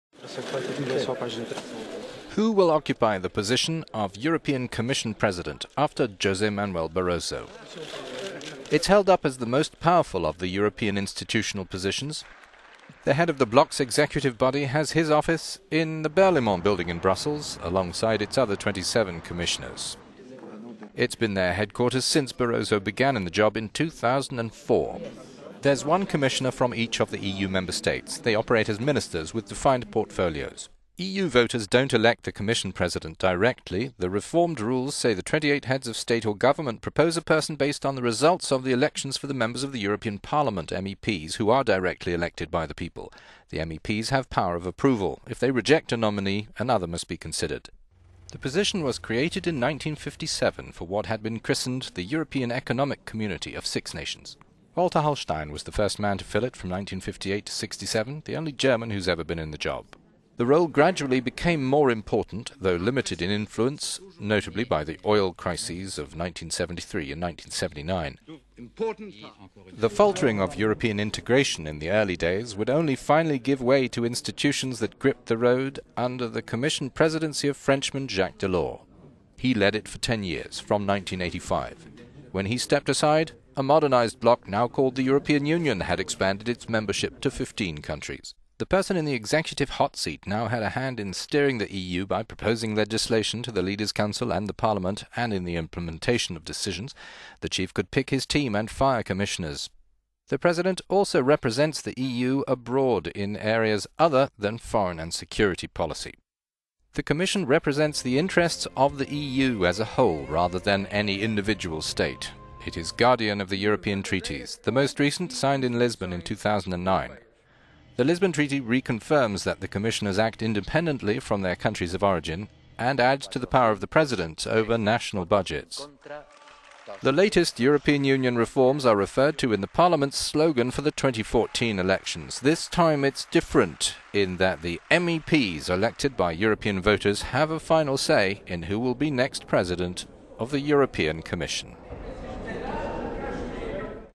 Rise and relativity of power in the EU executive body
After José Manuel Barroso, who will occupy the position of European Commission President? It is held up as the most powerful of the European institutional positions.

The head of the bloc's executive body and the other 27 commissioners have been headquartered in the Berlaymont building in Brussels since Barroso began in the job, in 2004.

There is one commissioner from each of the EU member states. They operate as ministers, with defined portfolios.

EU voters do not elect the Commission Presi...